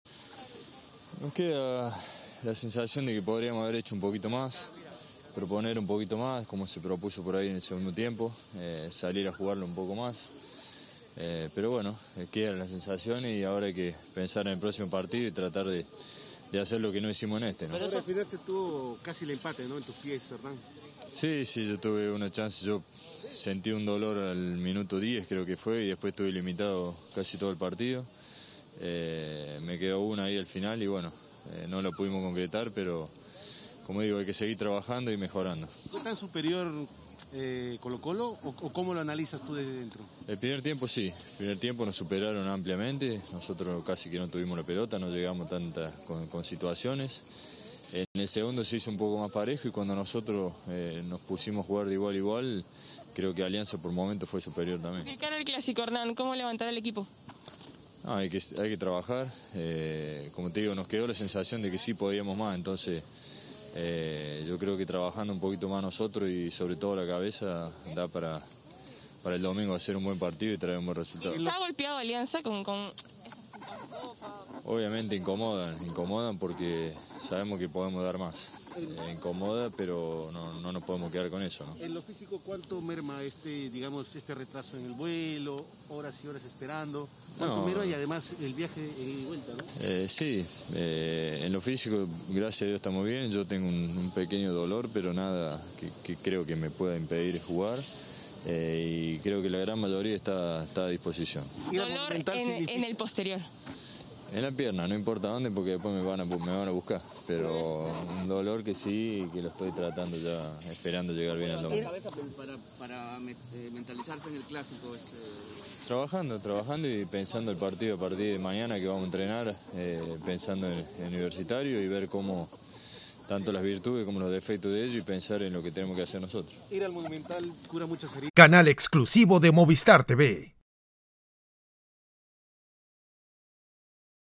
Hernán Barcos: "Tuvimos la sensación de que podríamos haber hecho más ante Colo Colo"
¡RECONOCE! 🗣 Hernán Barcos 🎯🇦🇷, delantero de Alianza Lima, en GOLPERUNOTICIAS 📺: “Tuvimos la sensación de que podríamos haber hecho más. Hay que seguir trabajando para mejorar. En el primer tiempo, Colo Colo nos superó ampliamente”.